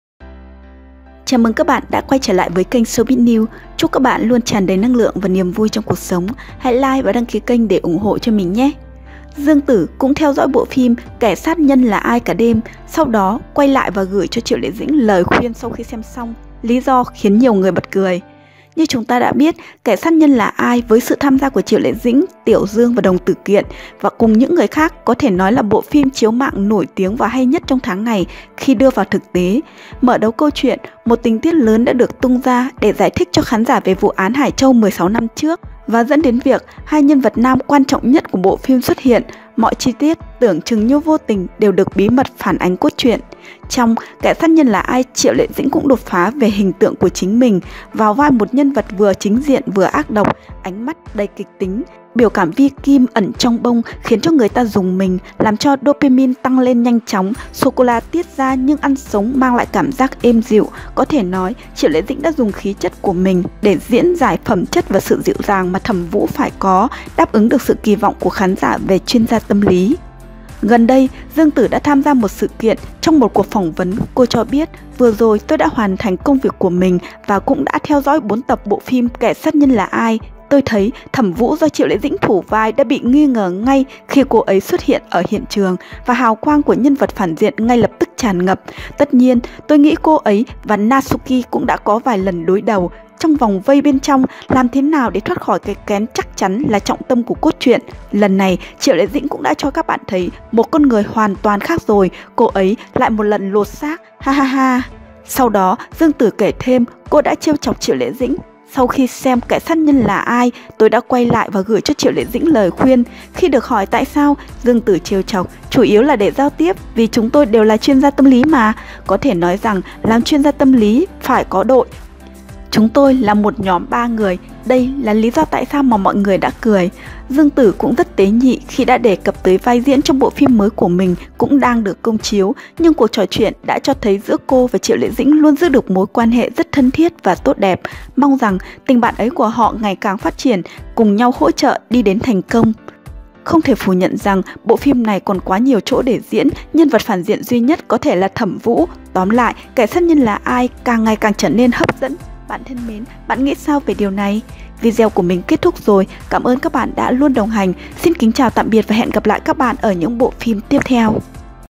Dương Tử: Triệu Lệ Dĩnh hoàn toàn lột xác trong "Kẻ sát nhân là ai?"
Gần đây, Dương Tử đã tham dự một sự kiện, trong một cuộc phỏng vấn, cô cho biết: Vừa rồi tôi đã hoàn thành công việc của mình và cũng đã theo dõi bốn tập phim "Kẻ Sát nhân là ai", tôi thấy, Thẩm Vũ do Triệu Lệ Dĩnh thủ vai đã bị nghi ngờ ngay khi Cô ấy xuất hiện ở hiện trường, và hào quang của nhân vật phản diện ngay lập tức tràn ngập, Tất nhiên, tôi nghĩ cô ấy và Natsuki cũng đã có vài lần đối đầu. Trong vòng vây bên trong, làm thế nào để thoát khỏi cái kén chắc chắn là trọng tâm của cốt truyện, lần này Triệu Lệ Dĩnh cũng đã cho các bạn thấy một con người hoàn toàn khác rồi, cô ấy lại một lần lột xác. hahaha!